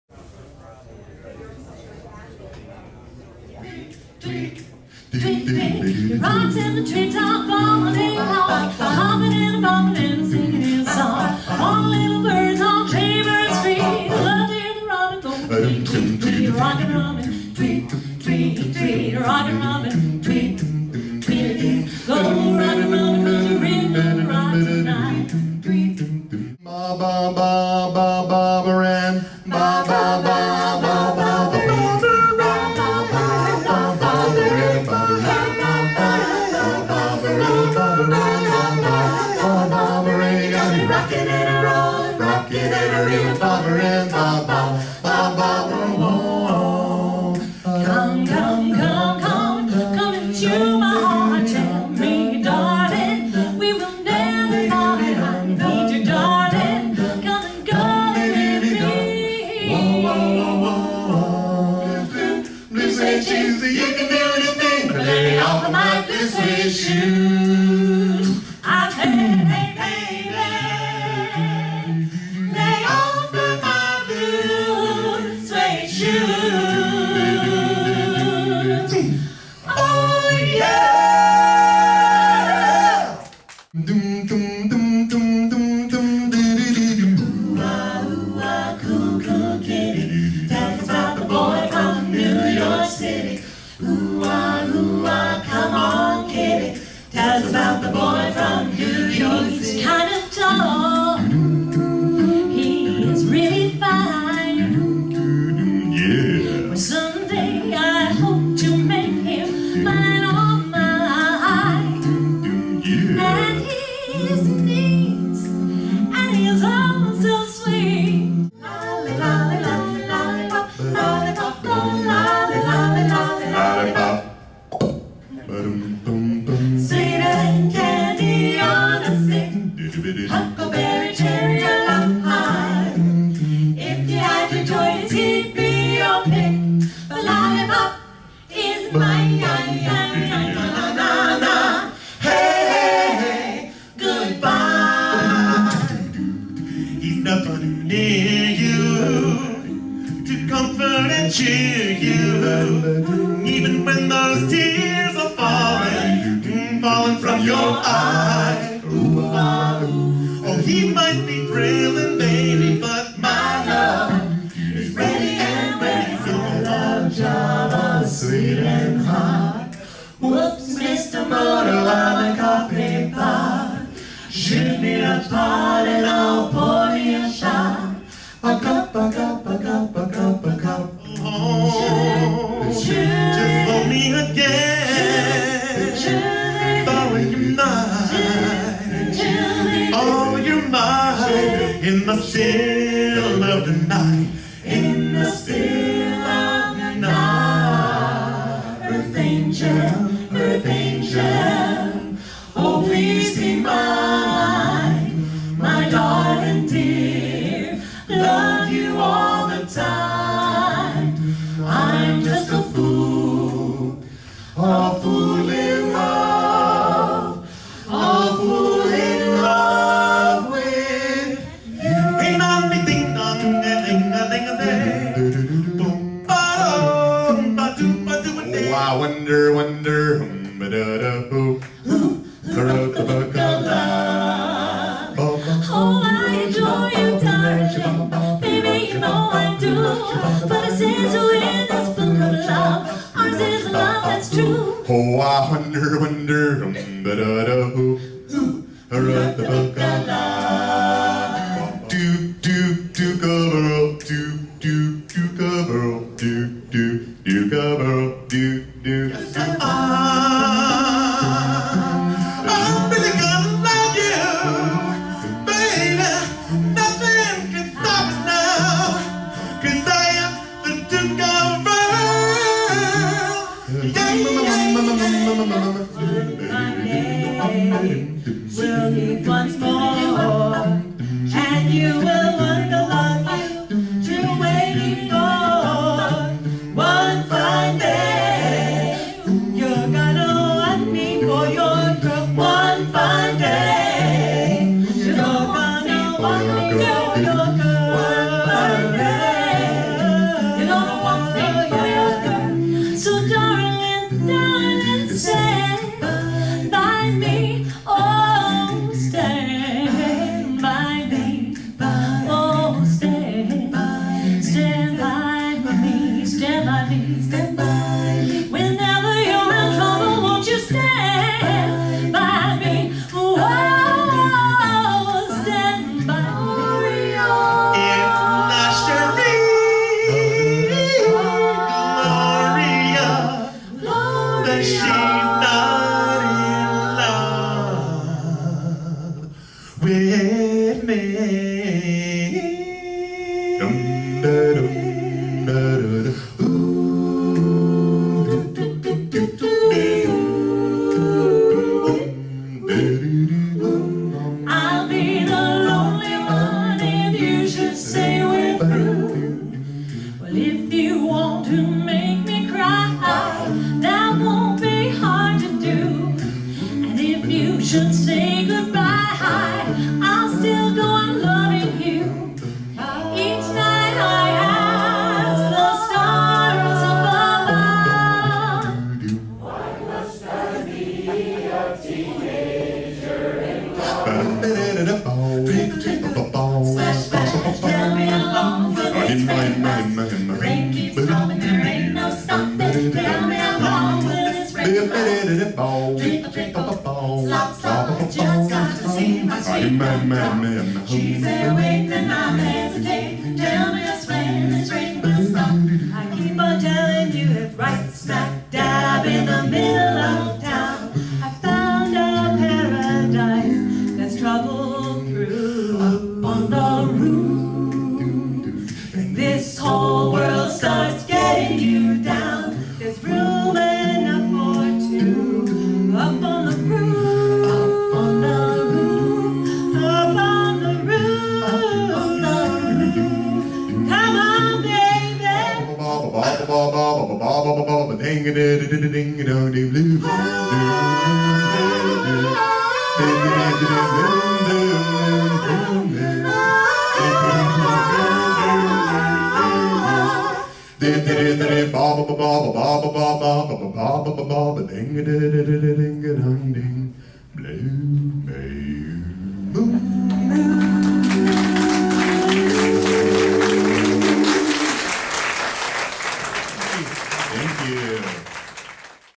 Harmony Express - Janet Hammer
Janet Hammer's group, Harmony Express, is based in San Diego.  Janet, Erin Ferguson, Don Bowman, and Jim Carkagis really kick a fun and entertaining show of doo wop standards. Video by Mark Danisovszky.